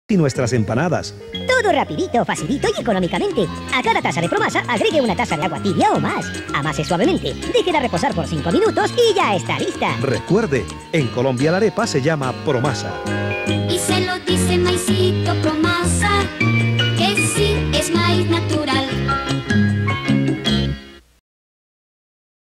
Promasa (Harina para arepas)
Este trabajo es de 1985. Lo hice en Fusagasugá, donde puse la productora después de mi separación en 1984. Allí me fui a vivir con mis 3 pequeños hijos, (aparecen en  una de las escenas) hicimos 6 o 7 comerciales hasta 1989, cuando nos trasladamos a Cali.